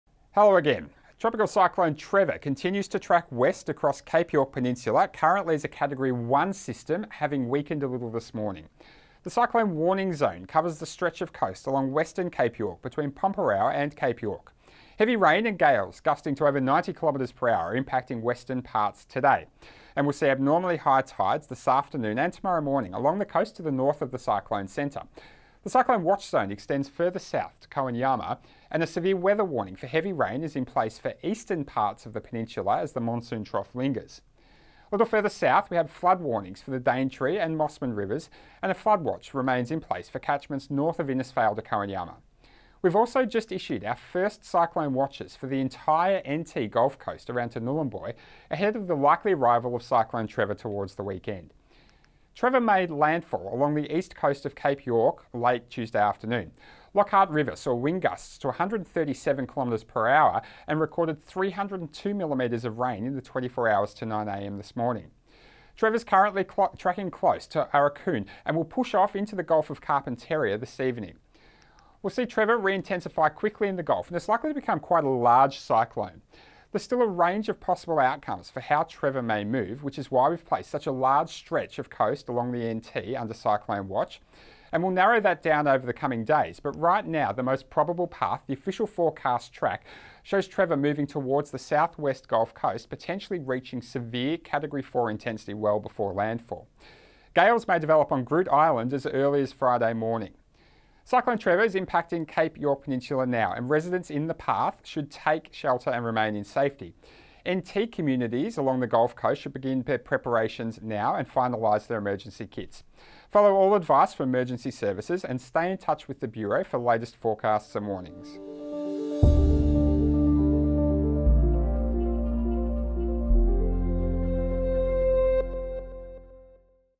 Severe Weather Update: tropical cyclone Trevor to move into the Gulf of Carpentaria, 20 March 2019
Category 1 tropical cyclone Trevor is currently located over the Cape York Peninsula and is slowly moving west. Video current at 12 pm AEST Wednesday 20 March 2019.

The cyclone will move into the Gulf of Carpentaria this evening, where it is expected to re-intensify rapidly to a Category 2 cyclone. At this stage the system is likely to cross the Northern Territory coast on Saturday as a Category 4 severe tropical cyclone.

Know your weather. Know your risk. Check for the latest cyclone advice and warnings, and follow advice from your local emergency services.